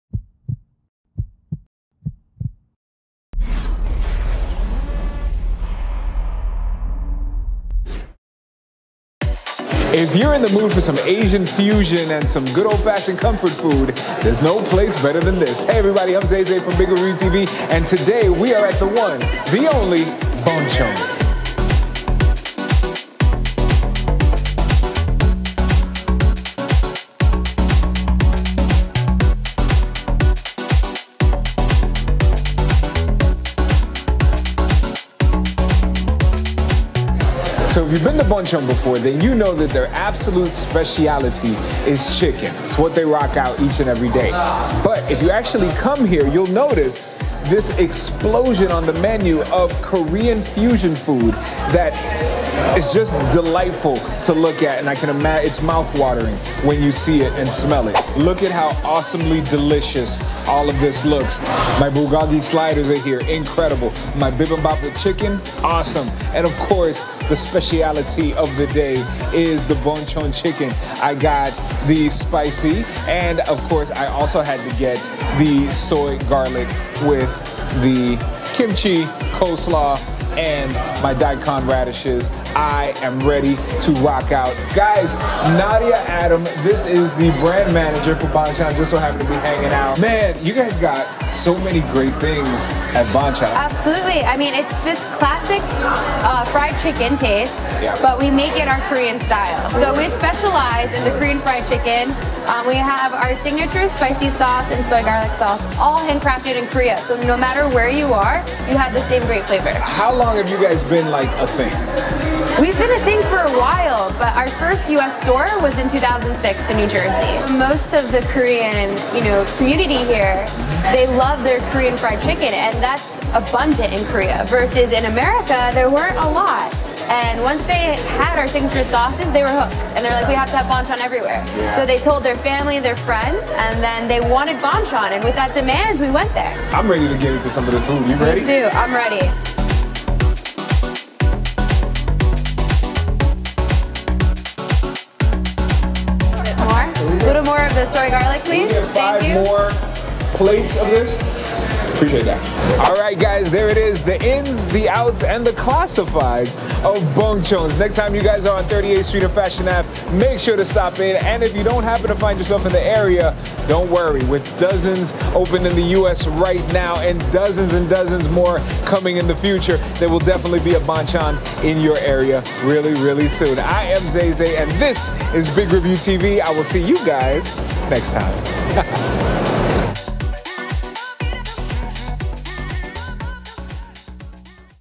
Bonchon a Restaurants in New York serving Korean Food
Bonchon has been called “The best chicken wings in the country” by CNN. Bonchon Chicken is cooked-to-order, made with the highest quality ingredients with savory, secret sauces: soy garlic and spicy.

Bonchon Chicken
207 West 38th Street New York United States 10018